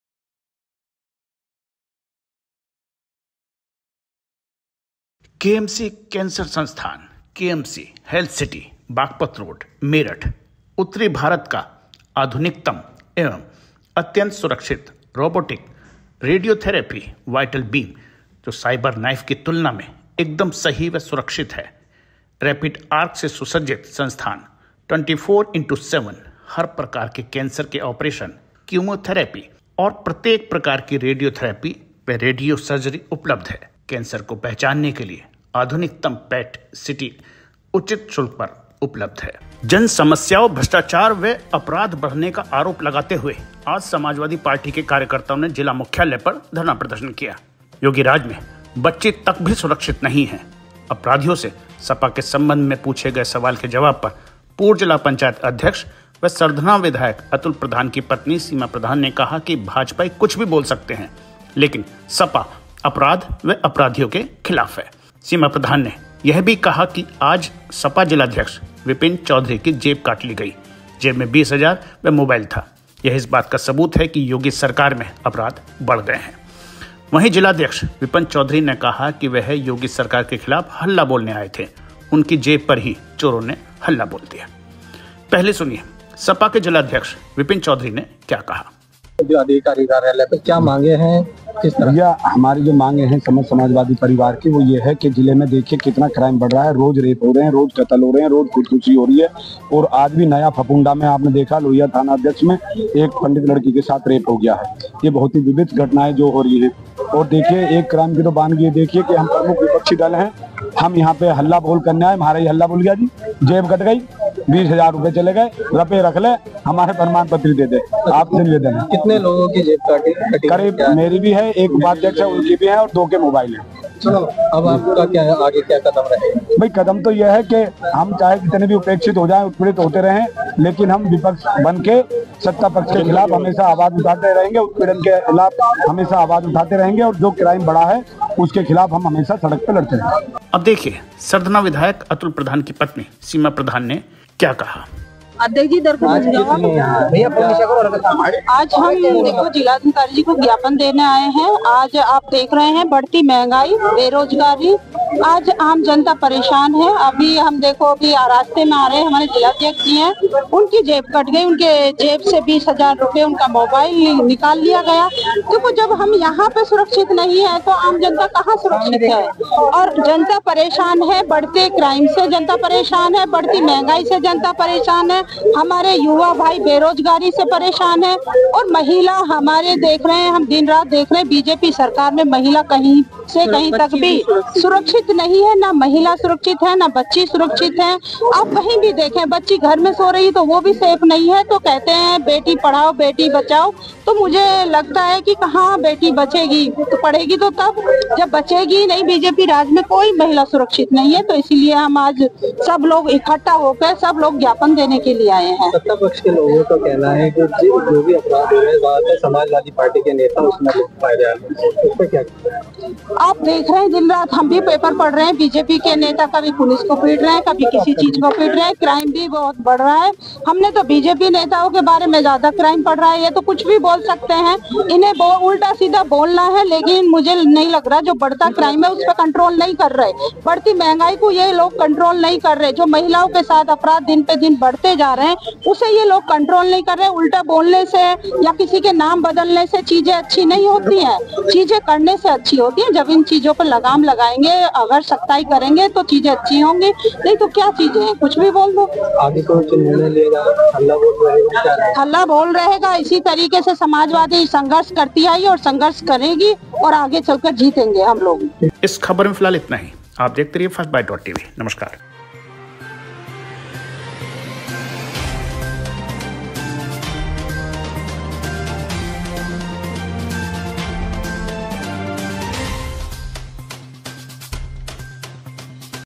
योगी सरकार के खिलाफ सपा का हल्ला बोल samajwadiparty, akhileshyadav ,cmyogi First Byte.tv
आये थे योगी सरकार के खिलाफ हल्ला बोलने,
जिलाध्यक्ष की जेब पर ही चोरों ने किया "हल्ला बोल",
जिलाध्यक्ष की जेब से बीस हजार व मोबाइल उड़ाया,
भ्रष्टाचार व अपराध पर अंकुश लगाने में सरकार फेल-सीमा,
आज घर में बच्ची भी सुरक्षित नहीं हैं-सीमा प्रधान,
जनसमस्याओं,भ्ष्टाचार व अपराध बढ़ने का आरोप लगाते हुए आज समाजवादी पार्टी के कार्यकर्ताओं ने जिला मुख्यालय पर धरना प्रदर्शन किया। योगी राज में बच्ची तक भी सुरक्षित नही हैं। अपराधियों से सपा के संबंध के बारे में पूछे गये सवाल पर पूर्व जिला पंचायत अध्यक्ष व सरधना विधायक अतुल प्रधान की पत्नी सीमा प्रधान ने कहा कि भाजपाई कुछ भी बोल सकते हैं, लेकिन सपा अपराध व अपराधियों के खिलाफ है। सीमा प्रधान ने यह भी कहा कि आज सपा जिलाध्यक्ष विपिन चौधरी की जेब काट ली गई, जेब में बीस हजार व मोबाइल था। यह इस  बात का सबूत है कि योगी सरकार में अपराध बढ़ गये हैं। जिलाध्यक्ष ने कहा था कि वह योगी सरकार के खिलाफ हल्ला बोलने आये थे, उनकी जेब पर ही चोरों ने हल्ला बोल दिया।

1.वाकई मोदीजी की तुलना नेहरूजी से नहीं की जा सकती